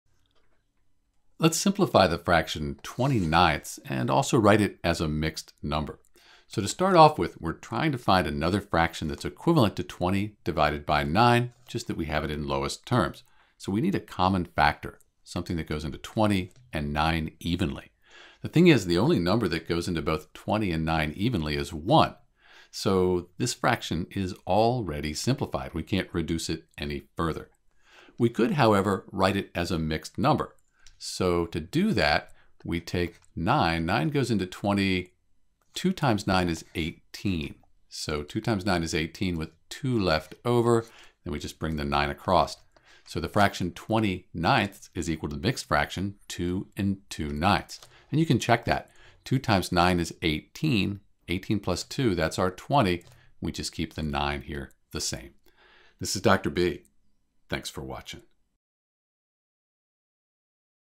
How to Simplify the Fraction 20/9 (and as a Mixed Fraction)
This means that we can simplify 20/9 to 20/9. The fraction is already simplified.  We can, however, write it as a mixed number which would be 2 2/9.